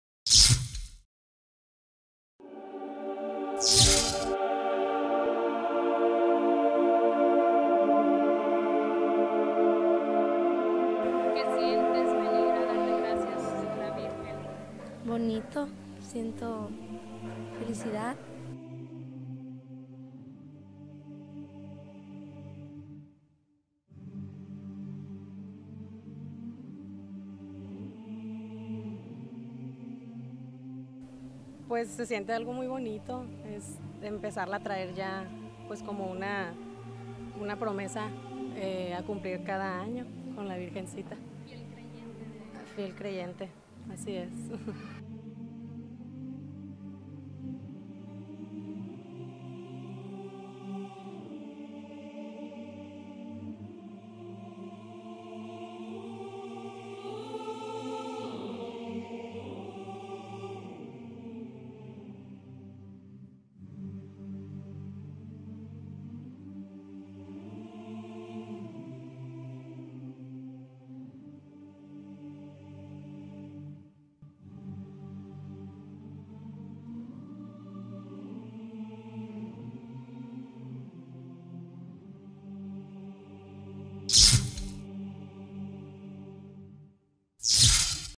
Devoción por la morenita, sigue vivá en los ahomenses
Cabe destacar que durante toda la noche de este lunes desfilaron decenas de feligreses que acudieron como todos los años al Santuario de Guadalupe, para darle gracias a la morenita por los favores recibidos y felicitarla por su día.